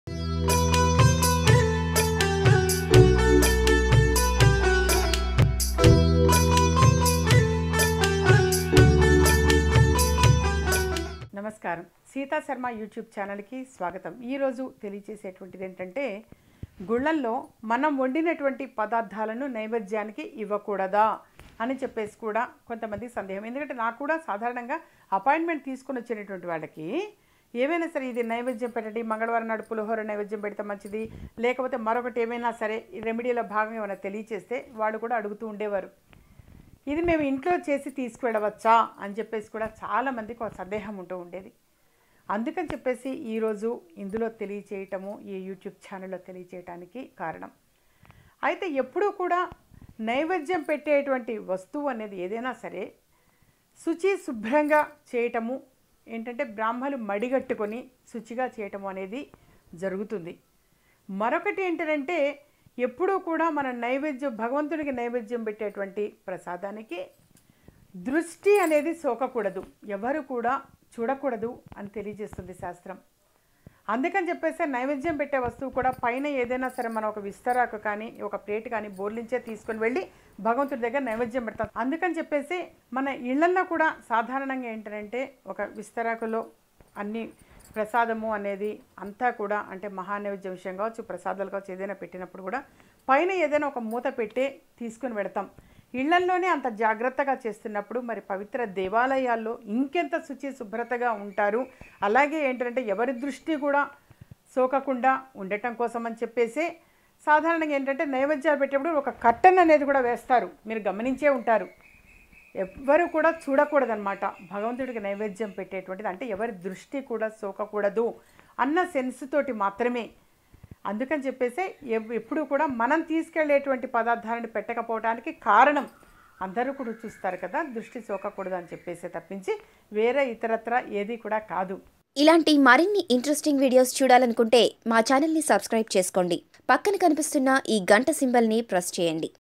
దేవునికి నైవేద్యం పెటేటపుడు తప్పకుండా పాటించవలసిన నియమాలు | Telugu Astrology Life Hacks
దేవునికి నైవేద్యం పెటేటపుడు తప్పకుండా పాటించవలసిన నియమాలు | Telugu Astrology Life Hacks - Vijayamargam by Sita Sarma. Telugu Videos for Better Life with Astrology. Tips for Better Living in Telugu.